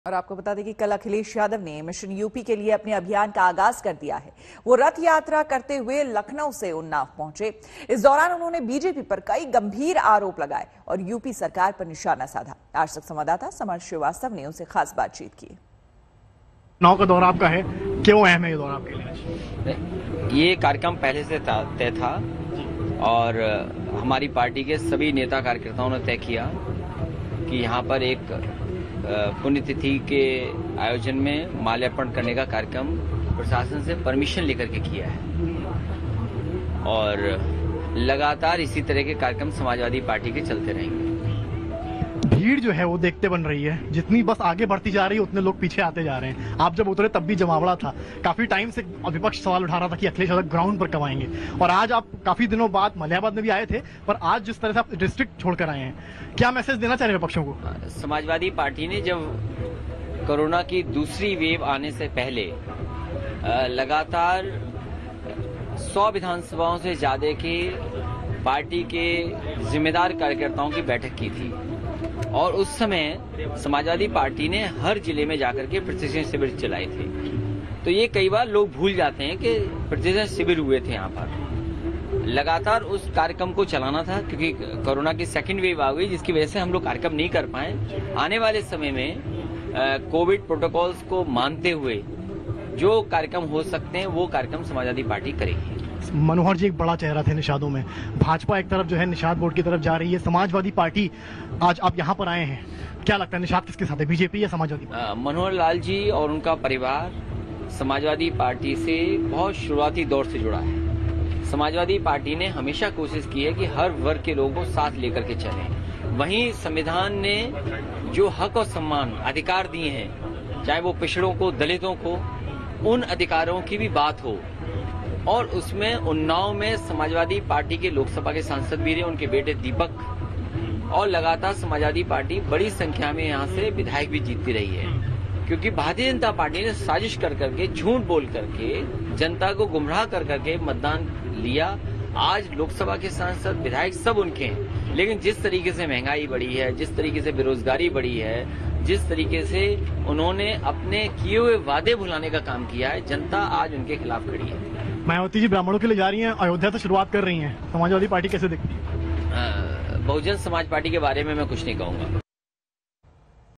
Akhilesh ने किया Mission UP के लिए आगाज, लखनऊ से उन्नाव तक की रथयात्रा
कल अखिलेश यादव ने मिशन यूपी के लिए अपने अभियान का आगाज कर दिया। वो रथयात्रा करते हुए लखनऊ से उन्नाव पहुंचे। इस दौरान उन्होंने बीजेपी पर कई गंभीर आरोप लगाए। और यूपी सरकार पर निशाना साधा। आजतक संवाददाता समर्थ श्रीवास्तव ने उनसे खास बातचीत की है।